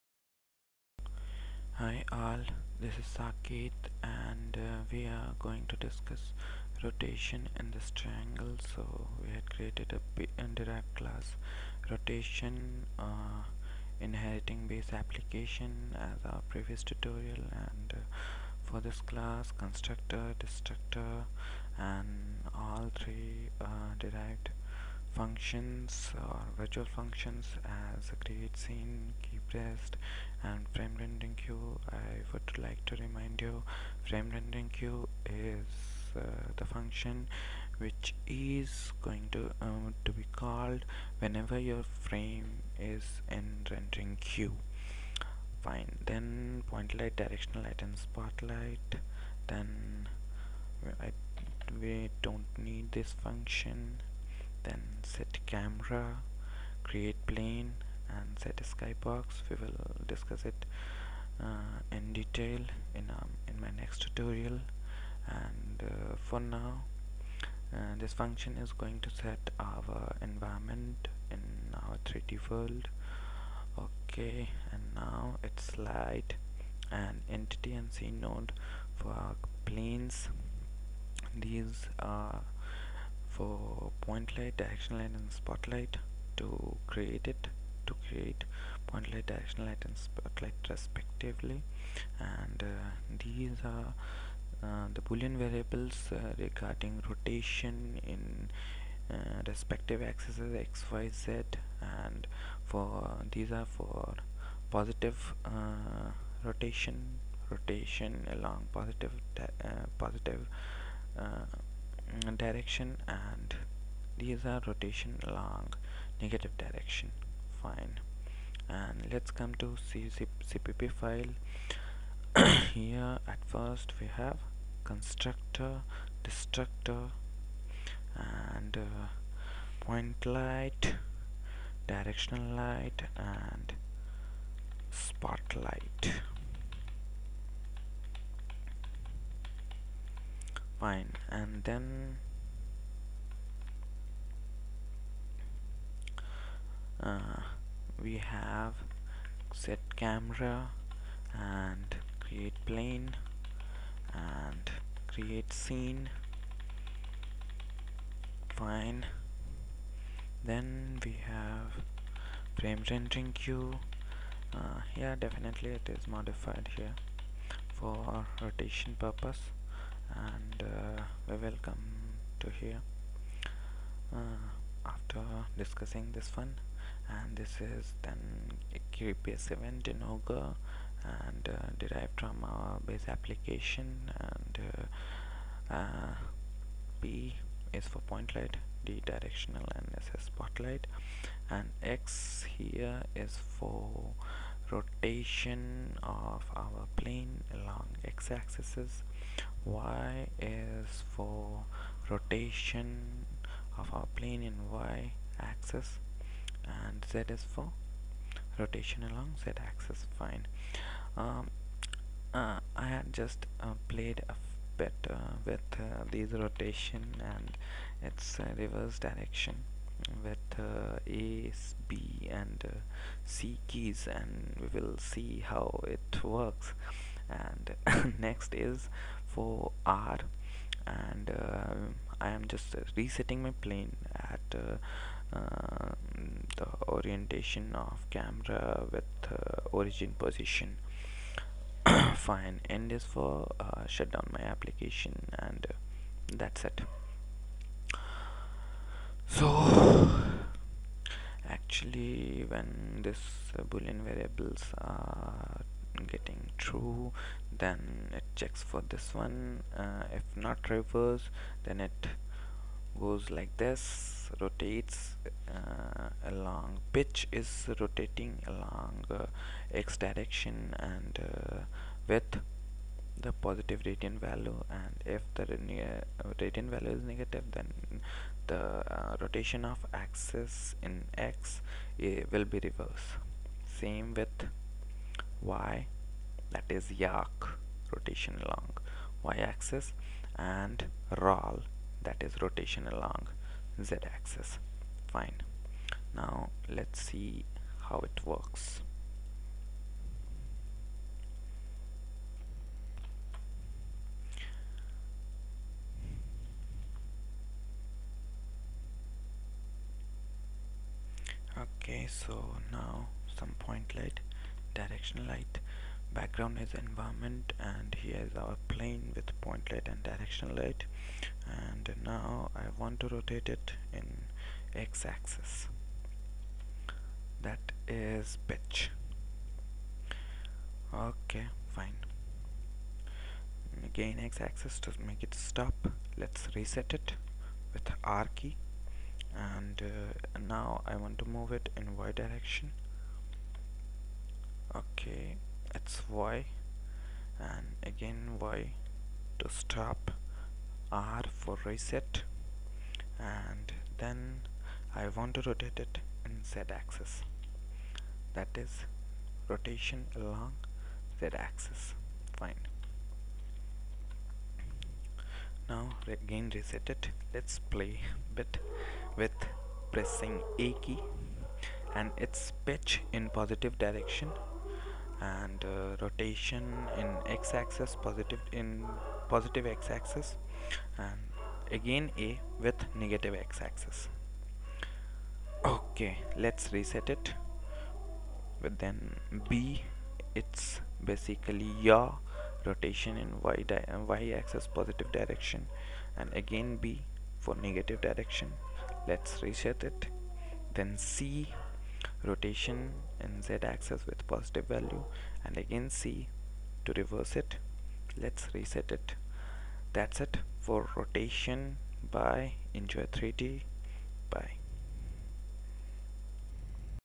Ogre3D Rotation along Axis X, Y and Z (Pitch,Yaw and Roll)
In Ogre3D, how to rotate 3D Models/Objects using key board with Spot lights, Directional light, Point light, and environment mapping.